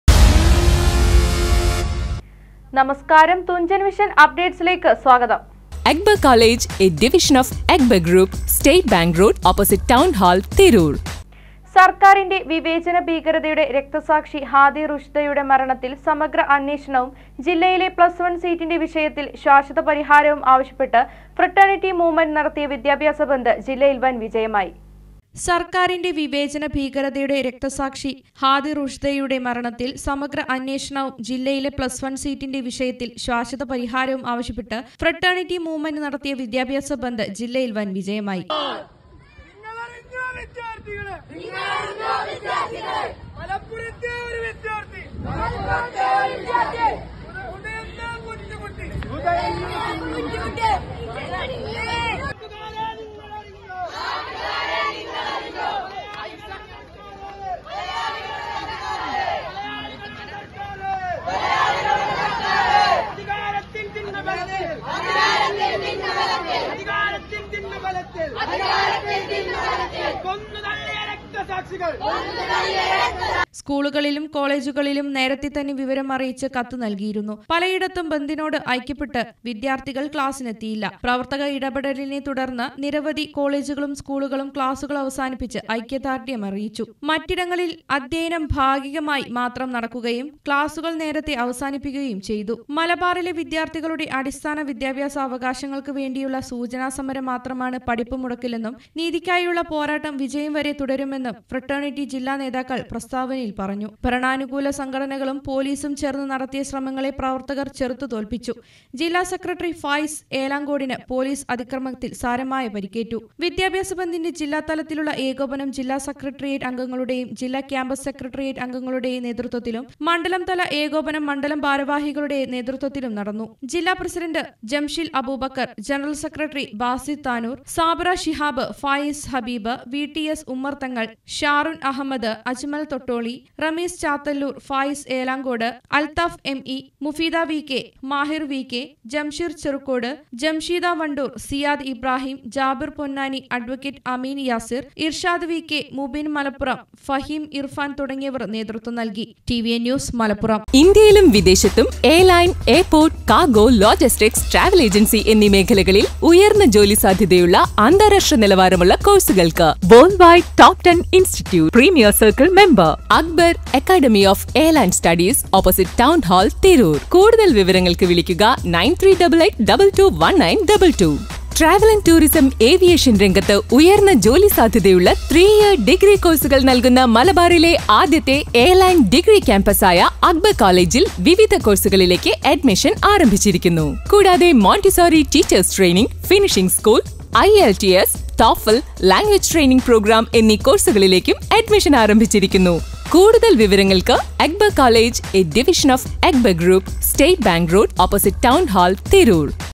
വിദ്യാഭ്യസ ബന്ദ് സൂചന മാത്രം നടപടിയില്ലെങ്കിൽ പ്രക്ഷോഭം ശക്തിപ്പെടുത്തും: ഫ്രറ്റേണിറ്റി
സർക്കാറിൻ്റെ വിവേചന ഭീകരതയുടെ രക്തസാക്ഷി ഹാദിറുഷ്ദയുടെ മരണത്തിൽ സമഗ്ര അന്വേഷണവും ജില്ലയിലെ  പ്ലസ് വൺ സീറ്റിൻ്റെ വിഷയത്തിൽ ശാശ്വത പരിഹാരവും ആവശ്യപ്പെട്ട് ഫ്രറ്റേണിറ്റി മൂവ്മെൻ്റ്  നടത്തിയ വിദ്യാഭ്യാസ ബന്ദ് ജില്ലയിൽ വൻ വിജയമായി.